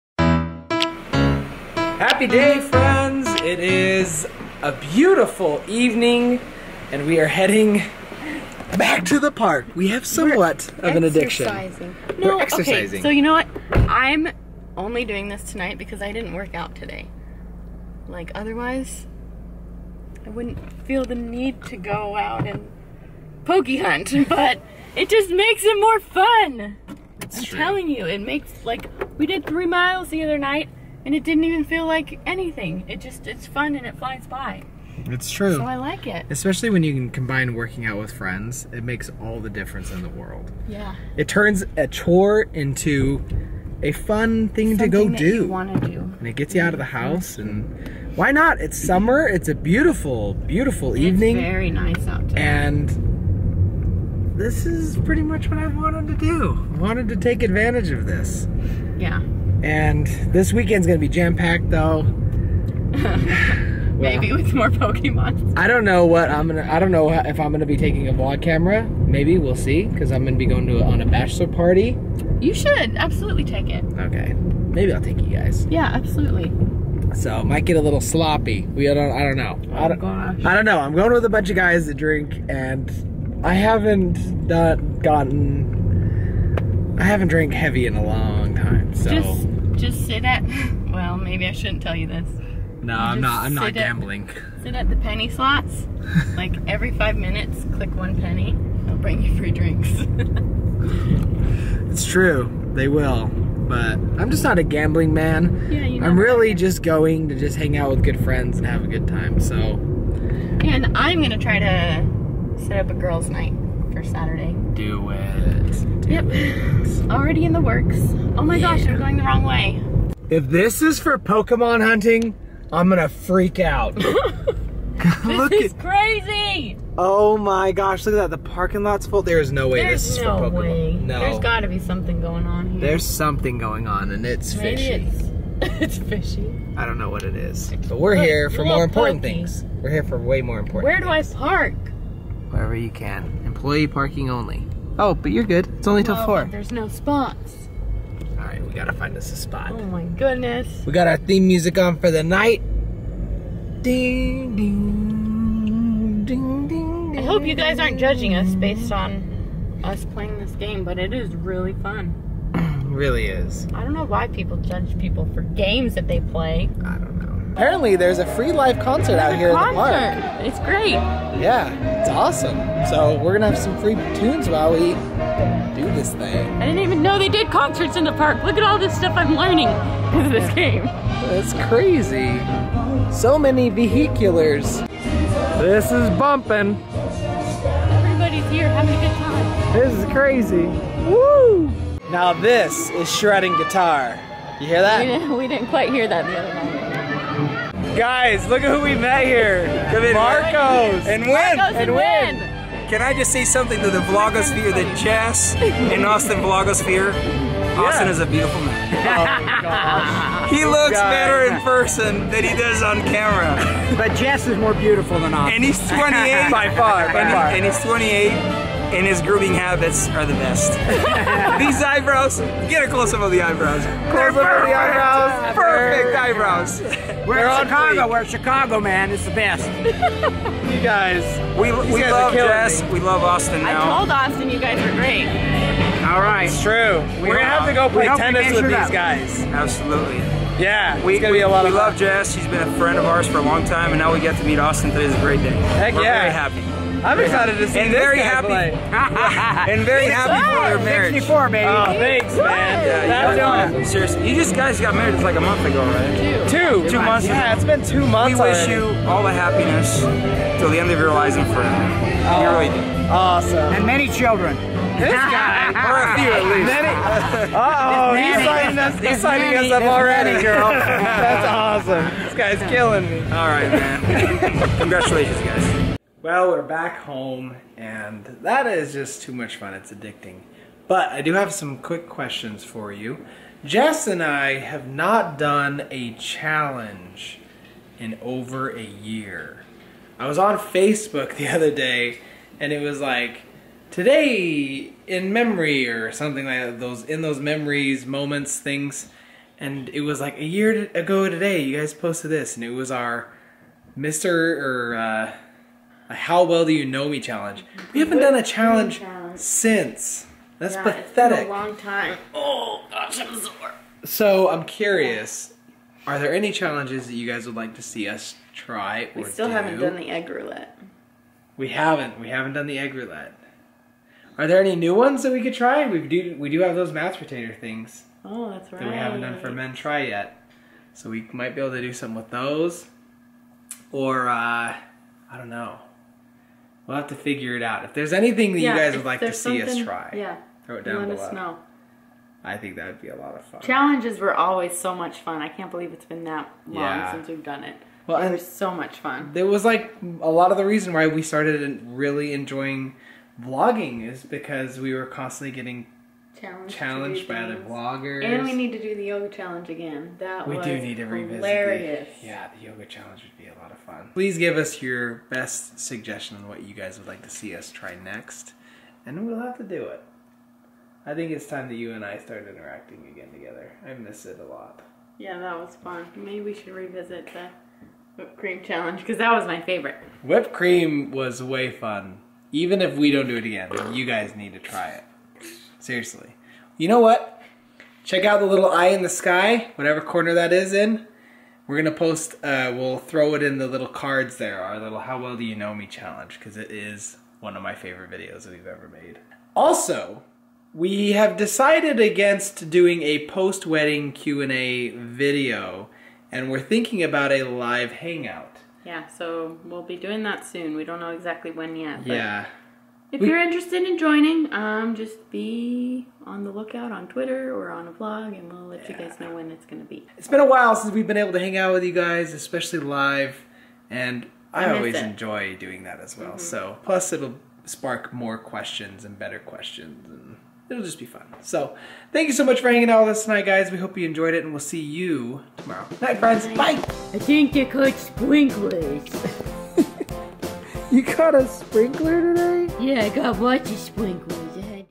What Are THEY Doing Here?
Pokemon Go is getting us outside and exercising more than we thought it would. The park is full of so many people and we don't know why! Austin finally gets to meet Jess' tennis friends, and we want you to challenge us!
⇨ NEW VLOGS EVERY WEEKDAY ⇦
⇨ FOLLOW US ⇦
Austin & Jess
⇨ T-SHIRTS ⇦
⇨ EQUIPMENT ⇦
Sony RX100 Mark III
GoPro Hero 3+ Black Edition
Canon S120